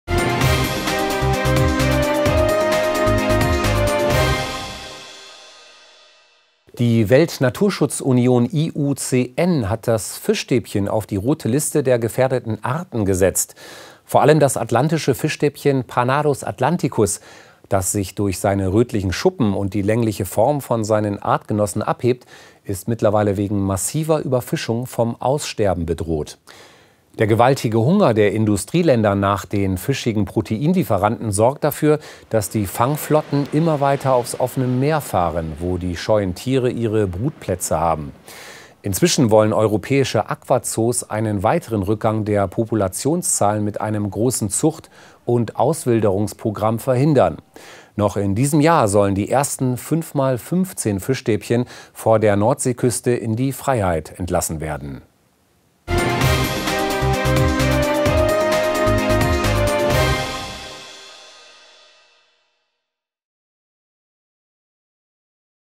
Fischstäbchen vom Aussterben bedroht [Postillon24]
Vor allem das Atlantische Fischstäbchen (Panadus Atlanticus), das sich durch seine rötlichen Schuppen und die längliche Form von seinen Artgenossen abhebt, ist mittlerweile wegen massiver Überfischung vom Aussterben bedroht.